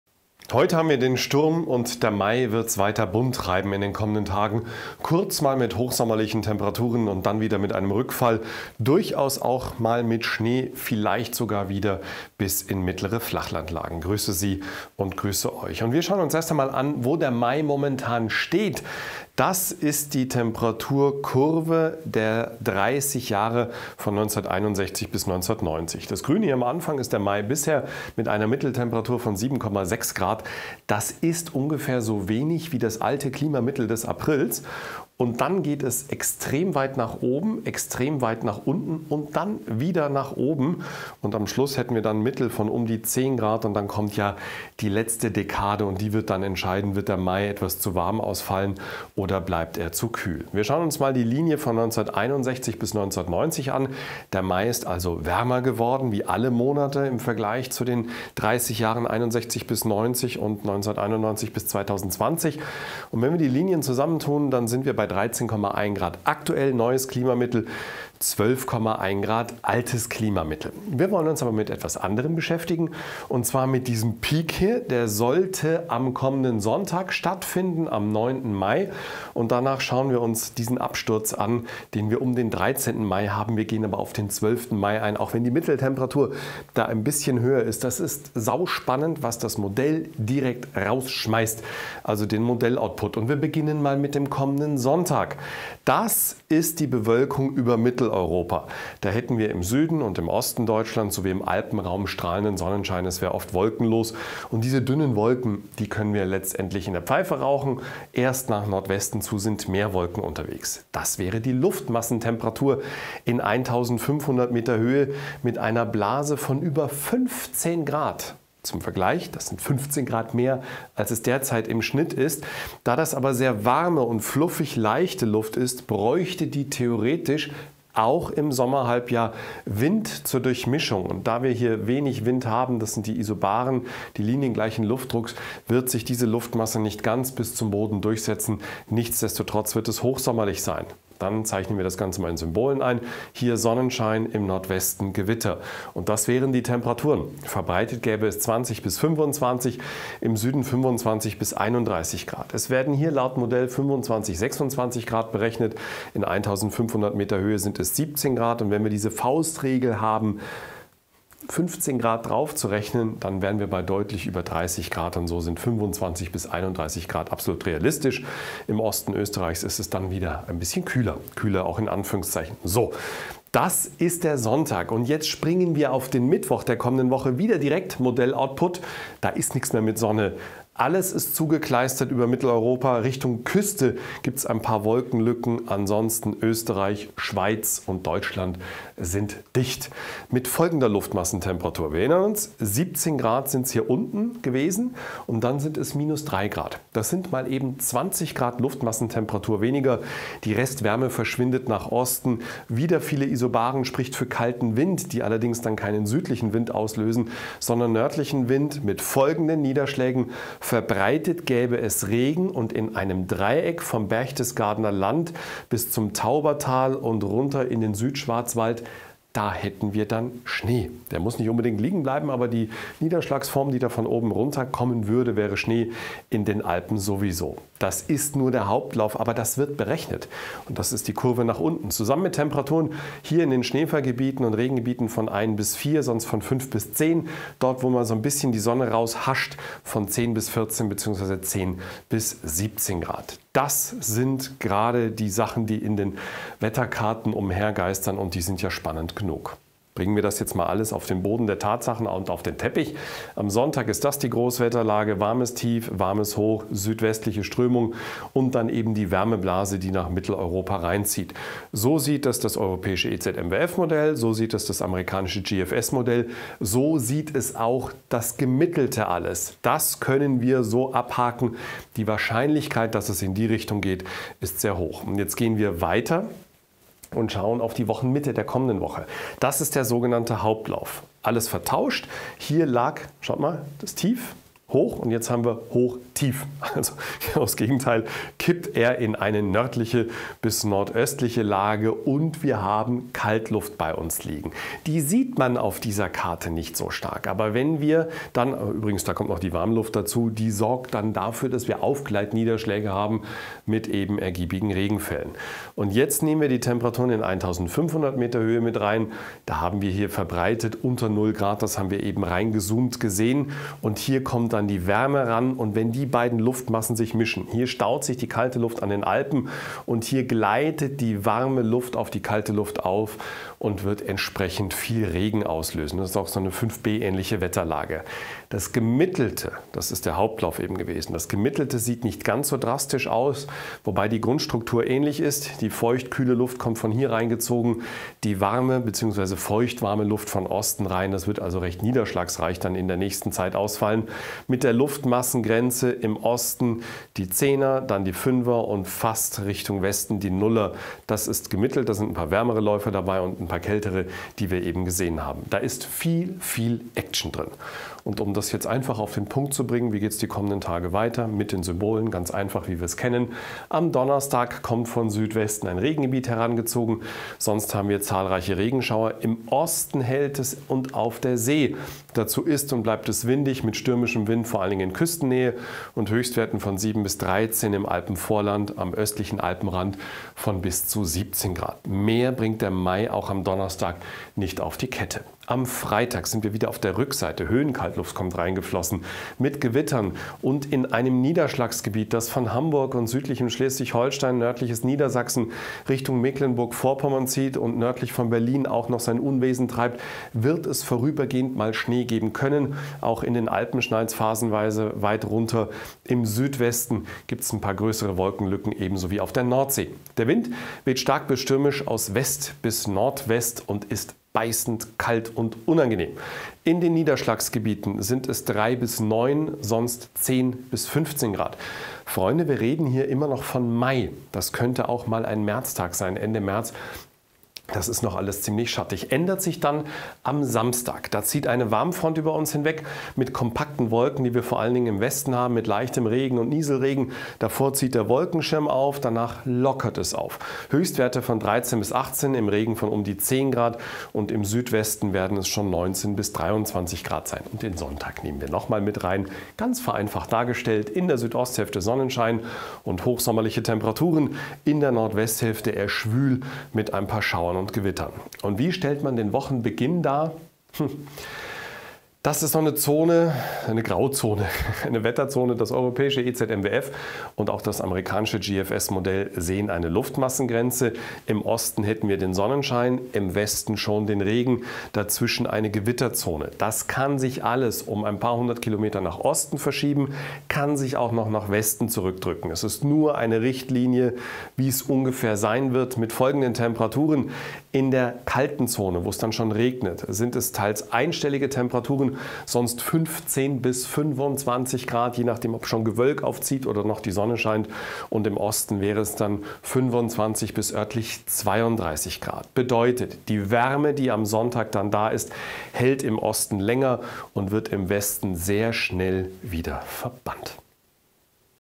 Nach Sturm Hitze, dann Regen (& Schnee?): Erste Hitzetage möglich. Mittelfristig unbeständig & nass.
Ein ungewöhnlich starker Mai-Sturm wütet gerade über Deutschland. Hinter diesem Sturm bleibt es bis Freitag ausgesprochen kühl, teils sogar kurz mit Schnee. Am Wochenende kommt der Switch auf deutlich wärmer, teilweise heiß. Rasch folgt von Westen ein neues Tief - dieses bringt erstens Gewitter und ergiebigen Regen mit einer Abkühlung und hat zweitens durchaus Überraschungspotential! Frau Holle soll ihre Finger im Spiel haben... ;)